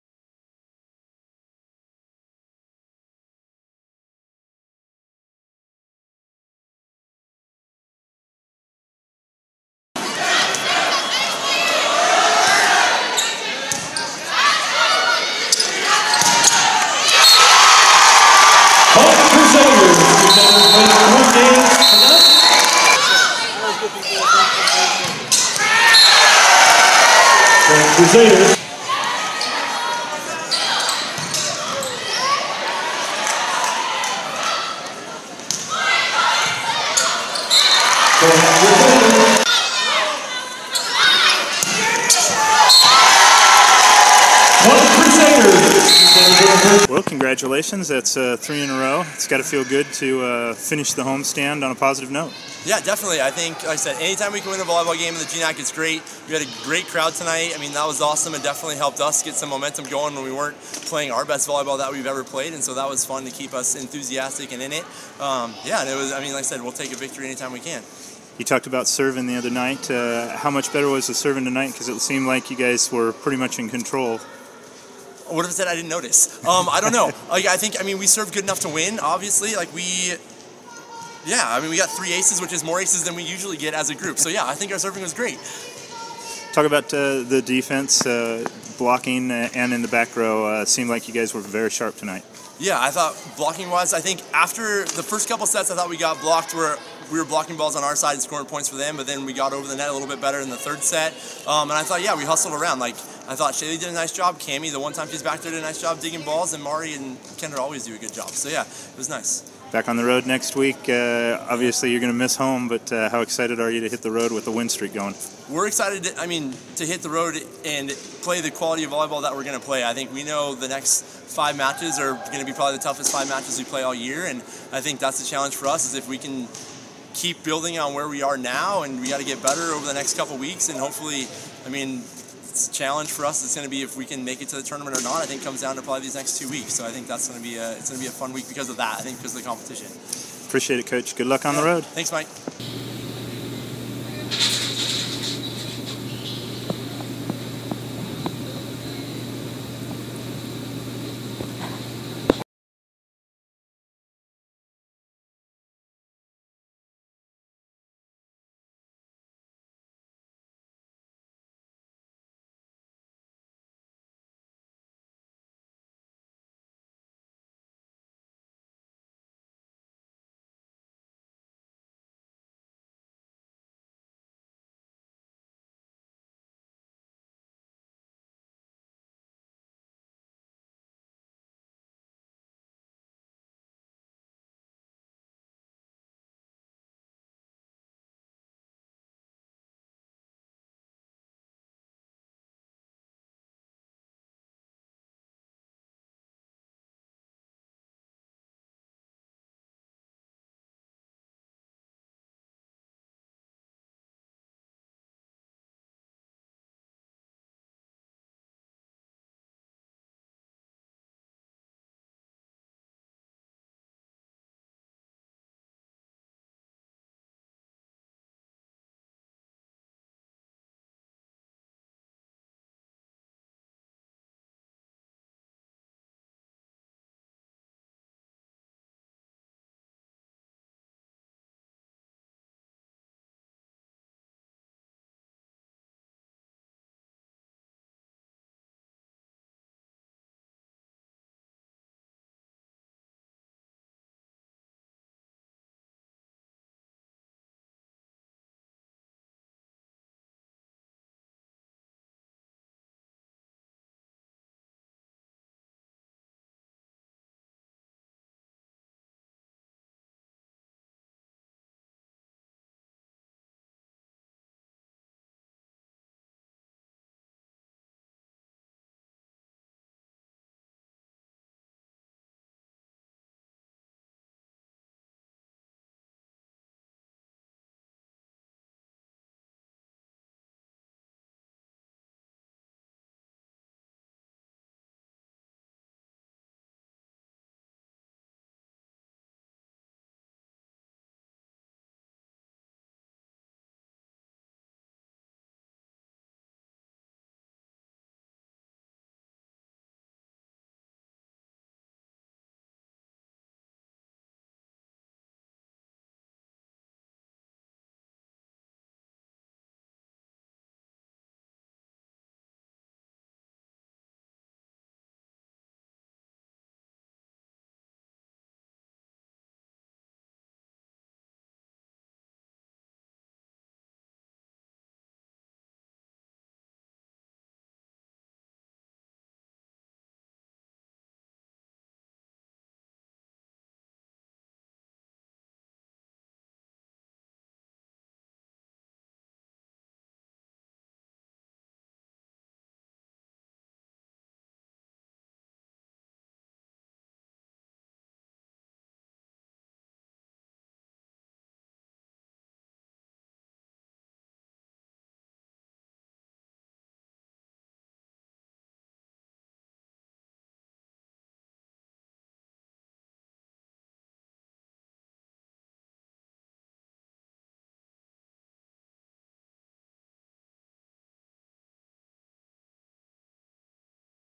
Crusader volleyball sweeps Saint Martin's
Highlights and post-match comments from head coach Doug English after the Crusaders sweep of Saint Martin's on Saturday.